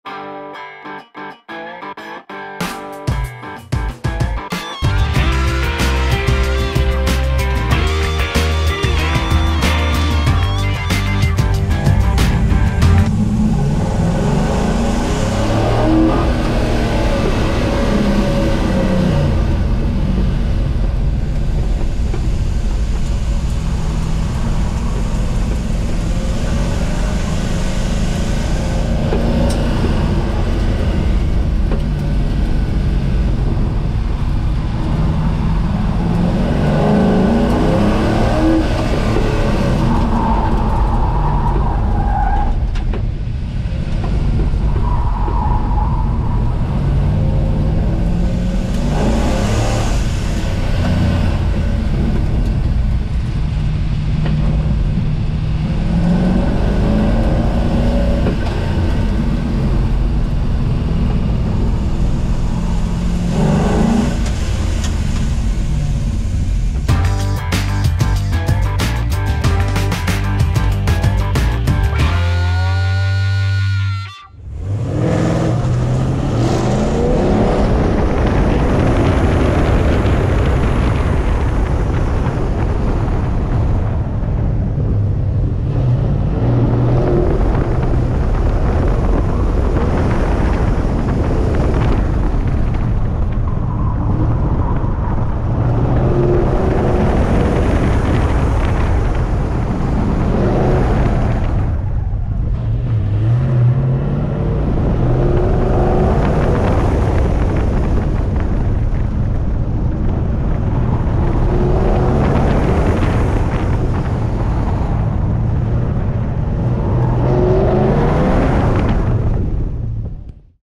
2020 Good Guys Southwest Nationals | Chevelle and F100
2020 Good Guys Rod and Custom Southwest Nationals in Scottsdale, AZ

1970 Chevrolet Chevelle - with experienced driver
1961 Ford F100 - with newbie driver